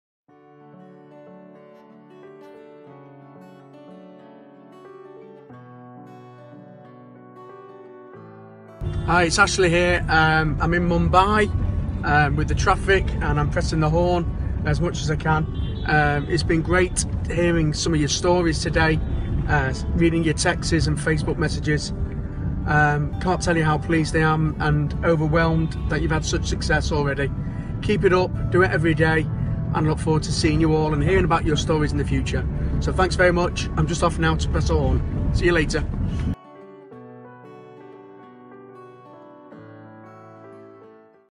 Dental Business Masters - Ashley Latter In Mumbai
One of best selling coaches in the UK, Ashley Latter on his trip to India for the Dental Business Masters Course for the Pioneer Batch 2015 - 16.